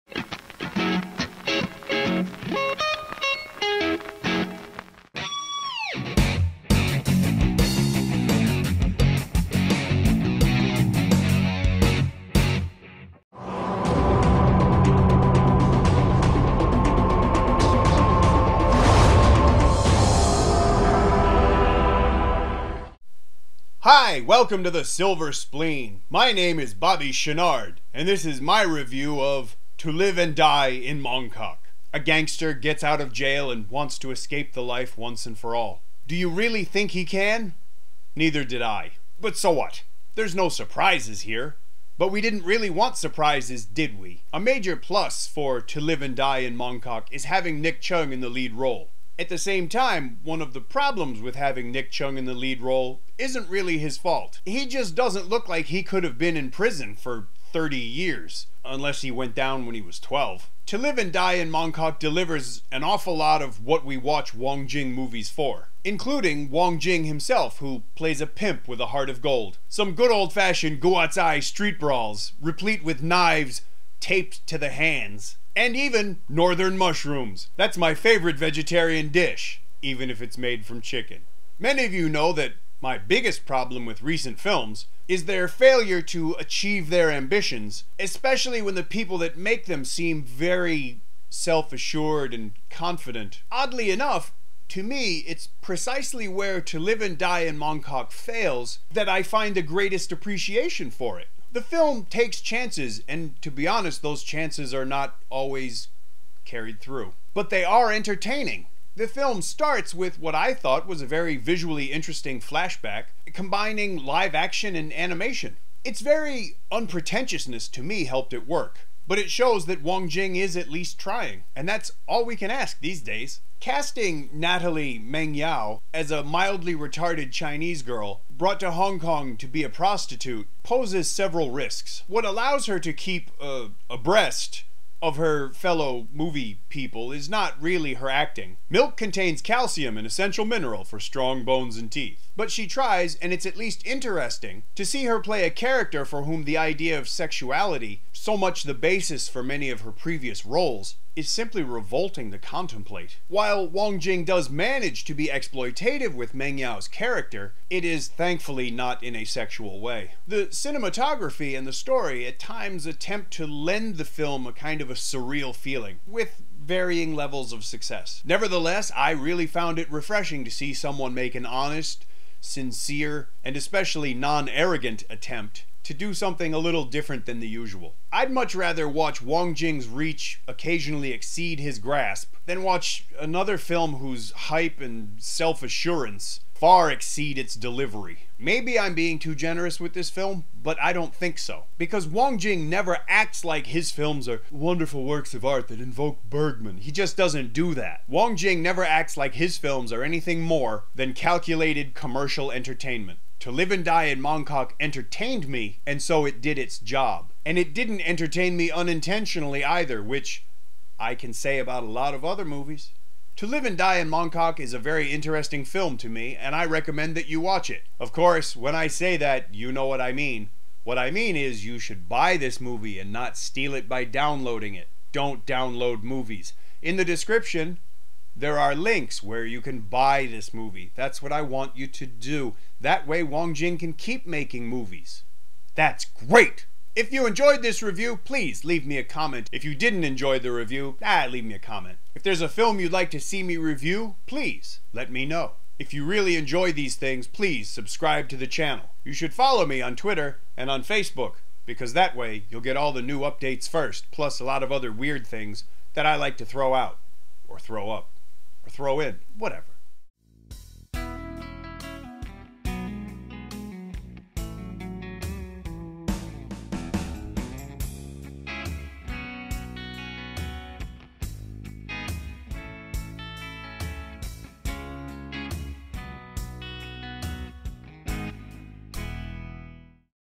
To Live and Die in Mongkok/旺角監獄 Movie Review
This is my review of To Live and Die in Mongkok starring Nick Cheung.

If you have any suggestions for films to be reviewed, please let us know.